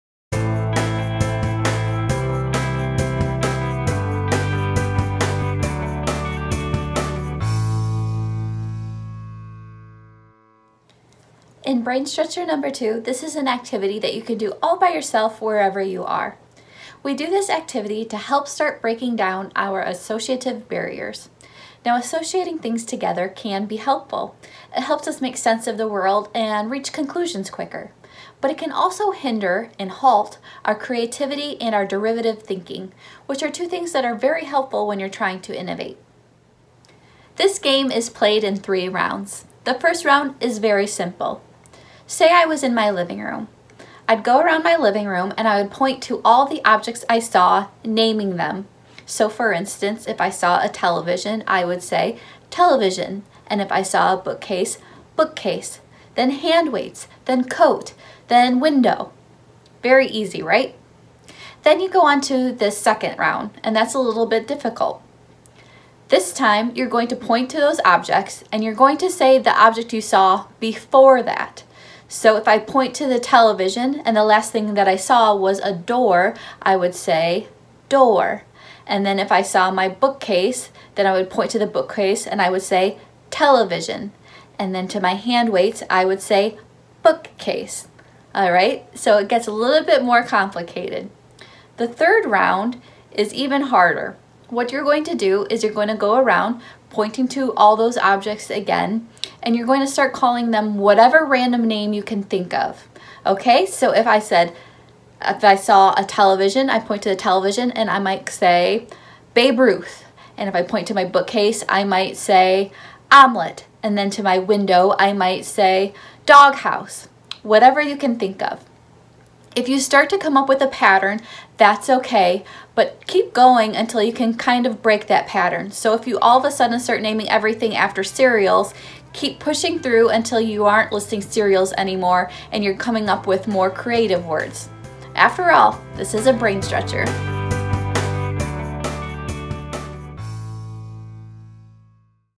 Brain Stretcher #2: Brain Chain
In this second activity of our Brain Stretcher series, Amy Gascon guides us through a three part activity naming objects right in your own home! The goal of this game is to break down associative barriers and stretch your brain a little. Have fun!